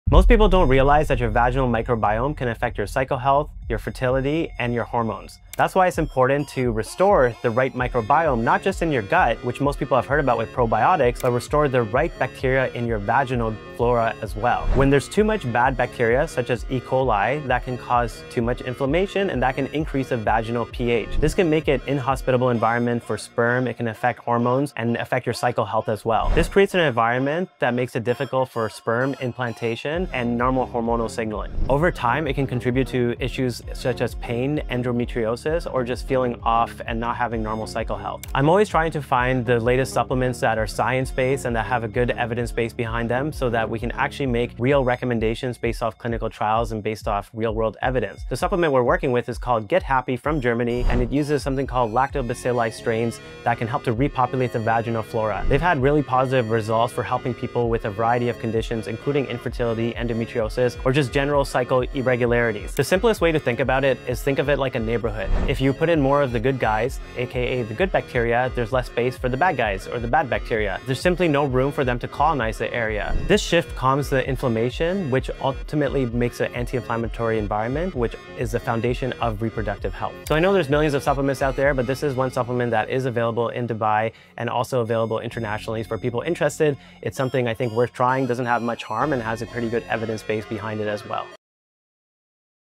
Restoring Fertility Through the Microbiome
Your vaginal microbiome plays a crucial role in fertility, hormone balance, and cycle regularity.
@gethappy.me helps repopulate it with the right bacteria, restoring the natural ecosystem that supports reproductive health.

Emerging research shows improvements in infertility, cycle irregularities, and even endometriosis symptoms when the vaginal microbiome is restored.